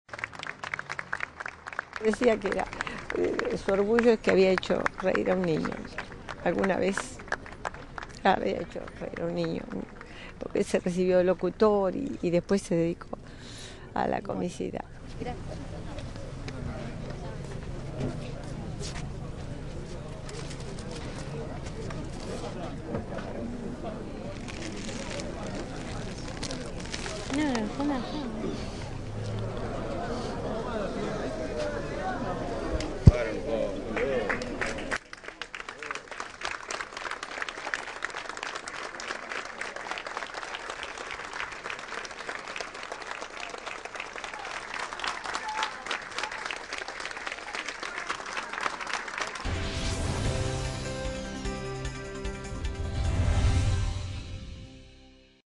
Último adiós a Juan Carlos Calabró en el cementerio de la Chacarita
En un marco de silencio y emoción familiares, amigos, allegados y fans del cómico se reúnen para despedir al creador de populares personajes televisivos como "El Contra", "Johnny Tolengo" y "Aníbal". El actor murió ayer a los 79 años.

Calabró fue velado desde las 16 de ayer en una cochería del barrio porteño de Palermo y por el lugar pasaron personalidades del mundo del espectáculo como Susana Giménez y Mirtha Legrand, entre más.